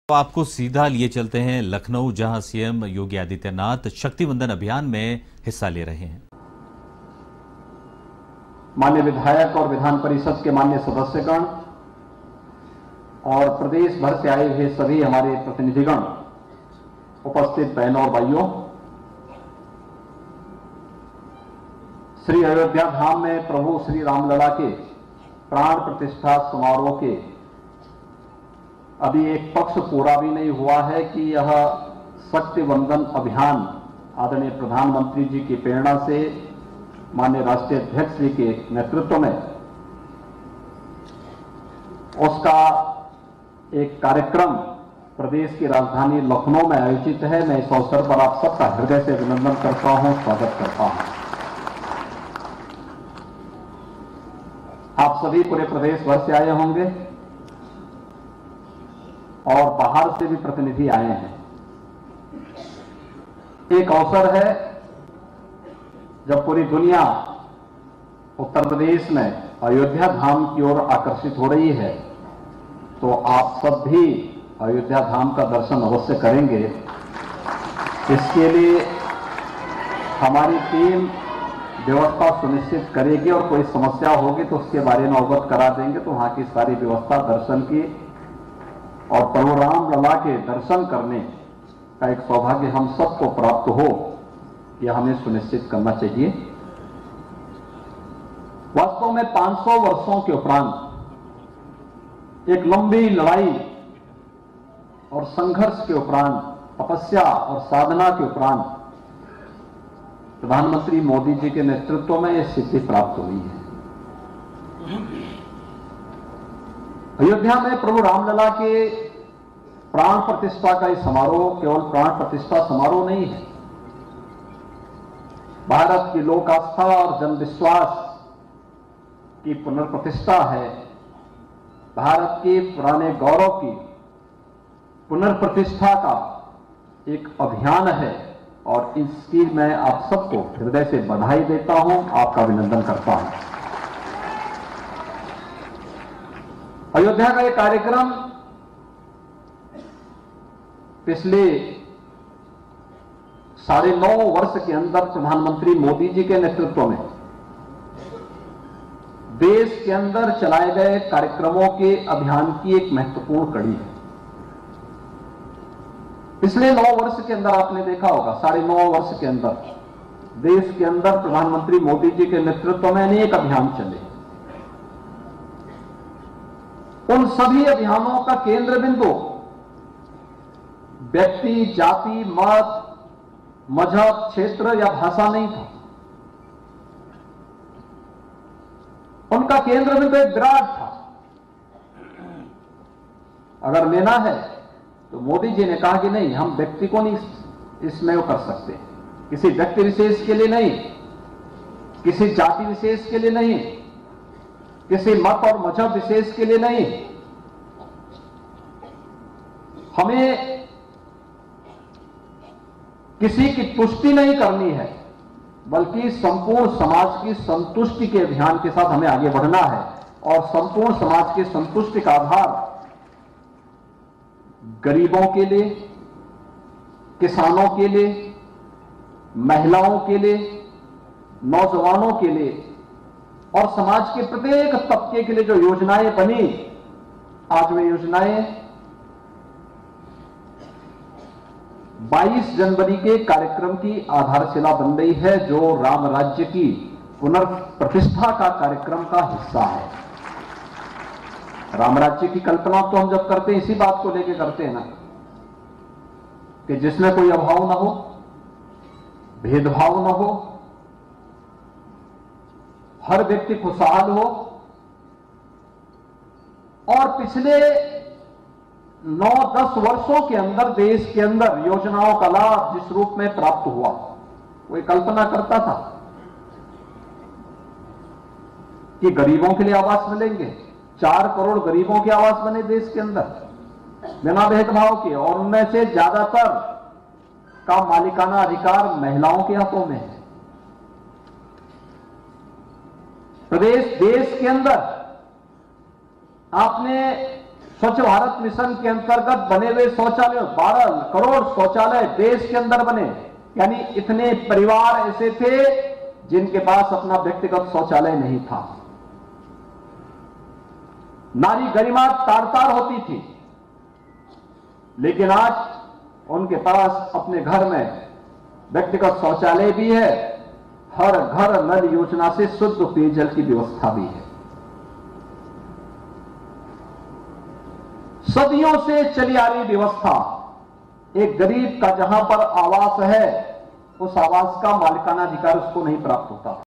CM Yogi Adityanath ने किया शक्ति वंदन अभियान का किया शुभारंभ | Lucknow | Uttar Pradesh | BJP
A1TV News Channel :

रविवार सुबह 8:30 बजे और शाम 4:30